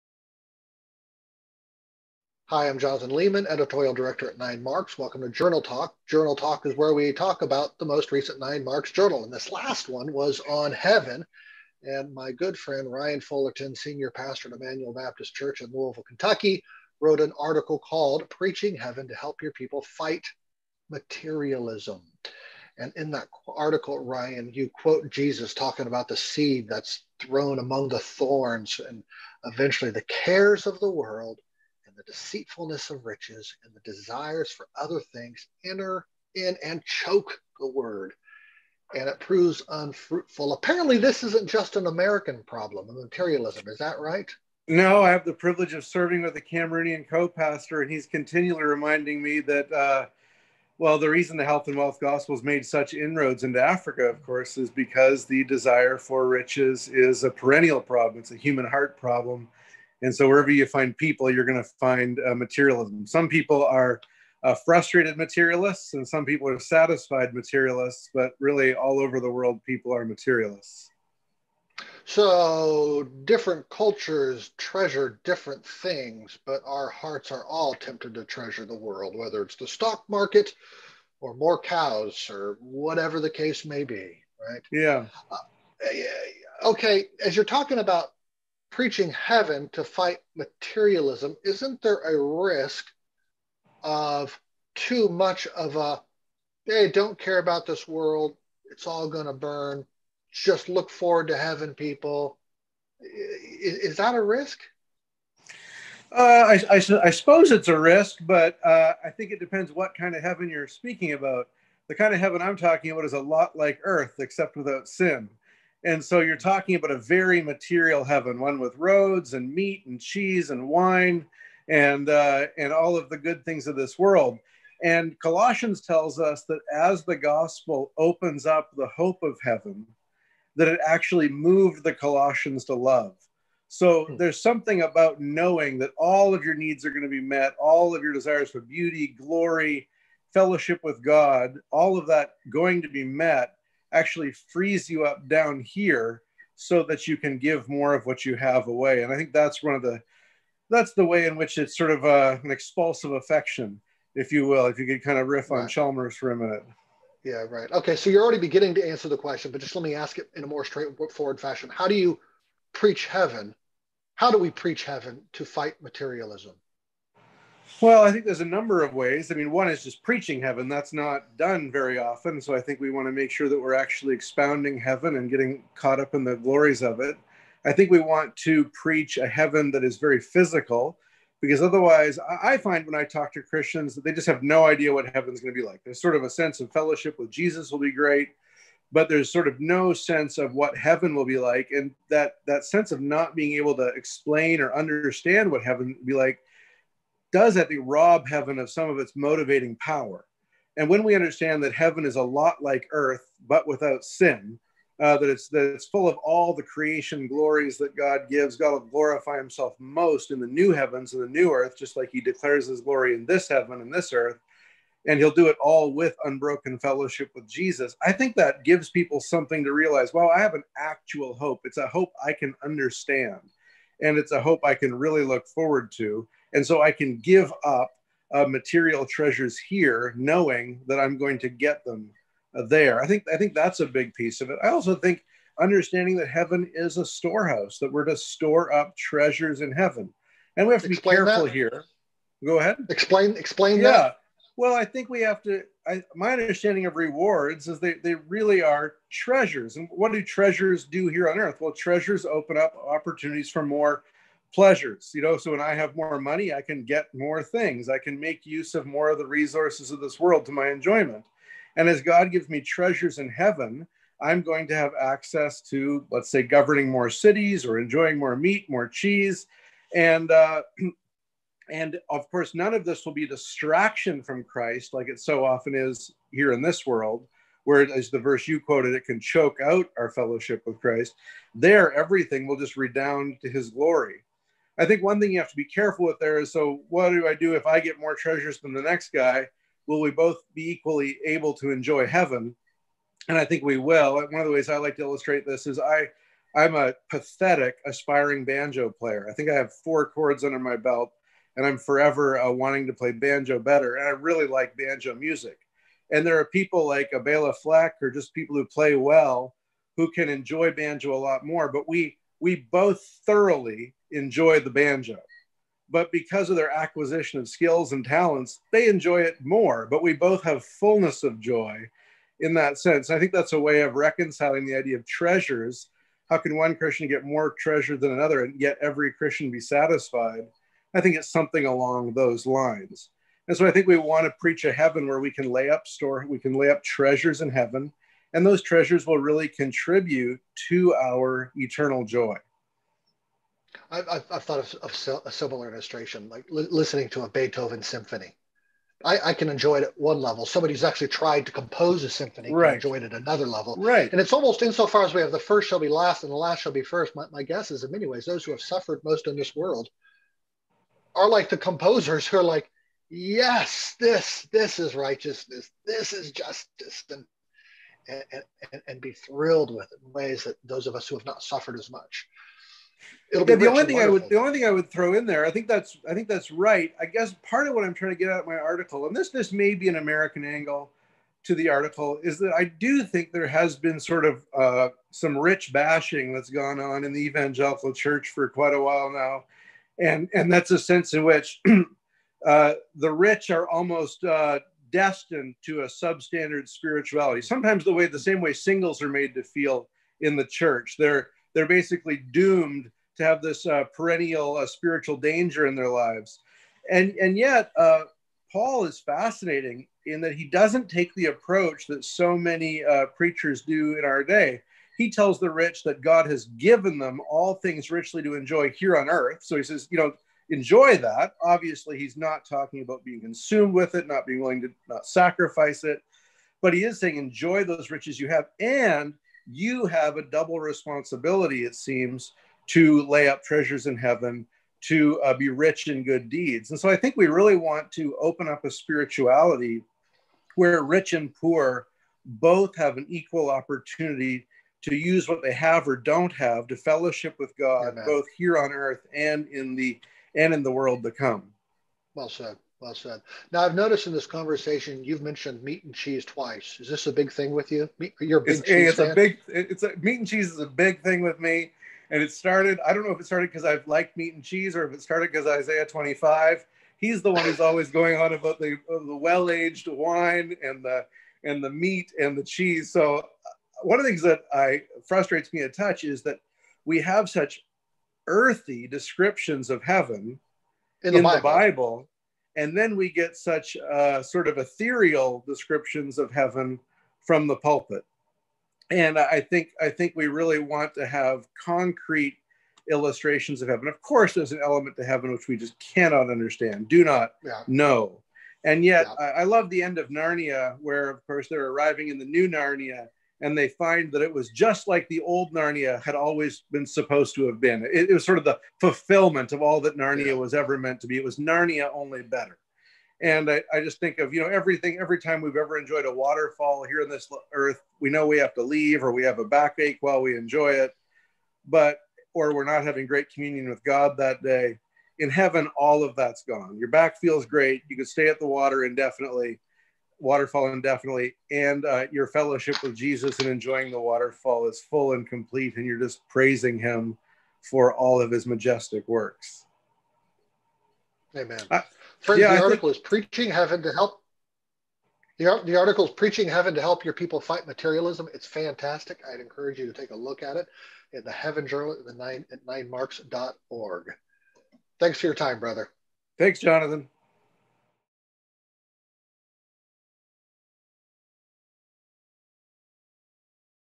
Preaching Heaven to Help Your People Fight Materialism (with Ryan Fullerton)
Download the free 9Marks Journal "Heaven: Rejoicing in Future Glory" on our